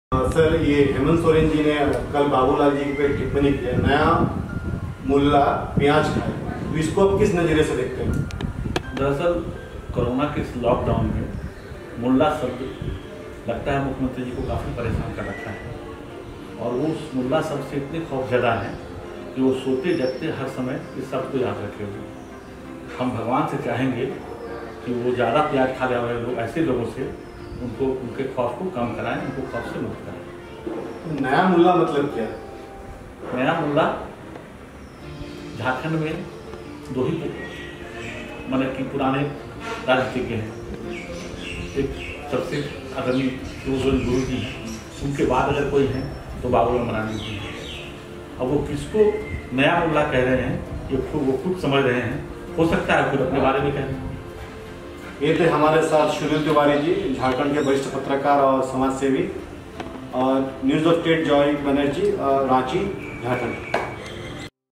वरिष्ठ पत्रकार सुनील तिवारी ने झारखंड के मुख्यमंत्री हेमंत सोरेन को बाबूलाल मरांडी के ऊपर ..
वरिष्ठ पत्रकार सुनील तिवारी ने झारखंड के मुख्यमंत्री हेमंत सोरेन को बाबूलाल मरांडी के ऊपर टिप्पणी करने पर खरी खोटी सुनाई